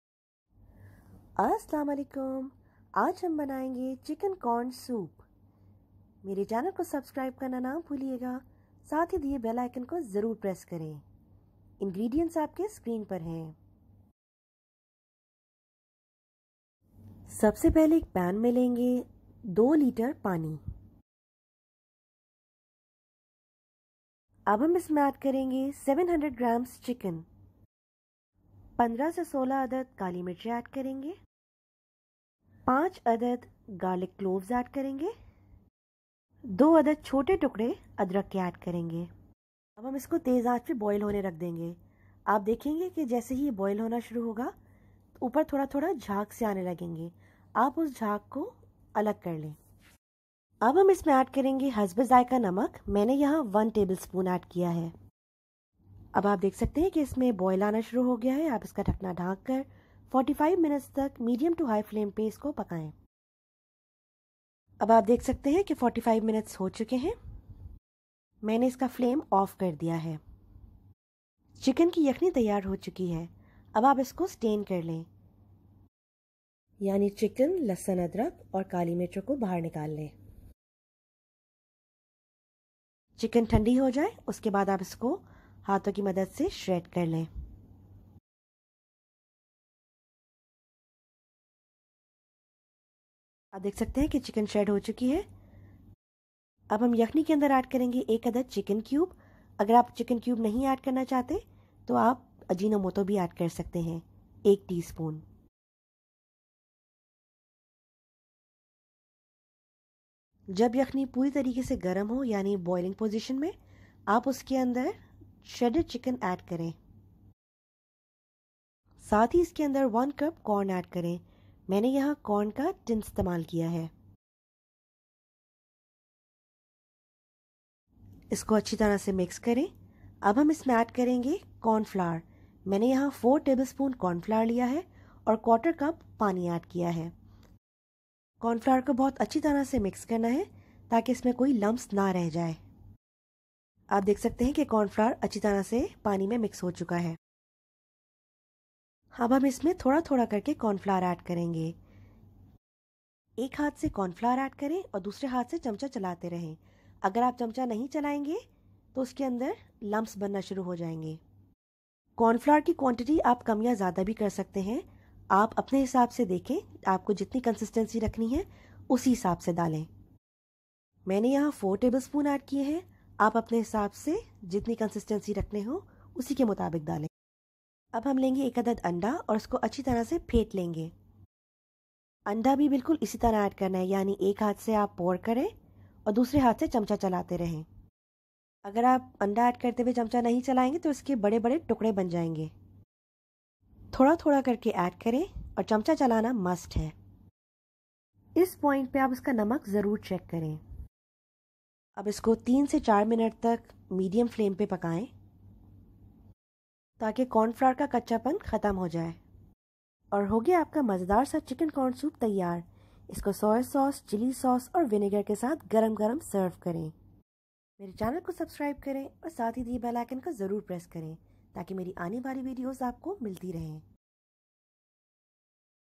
Chicken Corn Soup | How To Make Chicken Corn Soup | Easy Corn Soup | Easy Chicken Corn Soup Recipe
Ingredients:
Chicken 700 gram (boil with bones)
Water 2 Litres
Ginger Small Pieces 2
Salt To Taste (1 Tablespoon)
Chicken Cube or Ajino Moto 1 Teaspoon
Corn 1 Cup
Corn Flour 4 Tablespoon (can be changed according to your choice of thickness of soup)(mix in 1/4 cup water)
Egg 1

NOTE: Quantity of corn flour can be changed as per you choice.Make sure to keep stirring while adding the corn flour and egg.
If you don't want to add chicken cube you can use 1 teaspoon of Ajino Moto.
Do not hesitate to ask any question. Do give suggestions and feedback.Your feedback matters alot.